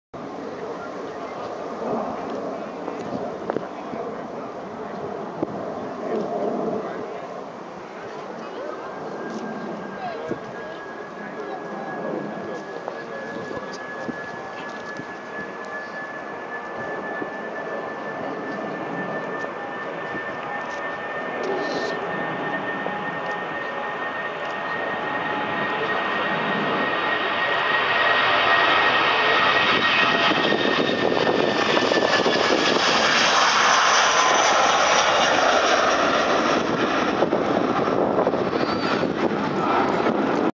British Aiways Airbus A319 take off Amsterdam Schiphol
British Airways
Airbus A319-131
G-EUPH (cn 1225)

taking off runway 36L -18R (Polderbaan )

Amsterdam Schiphol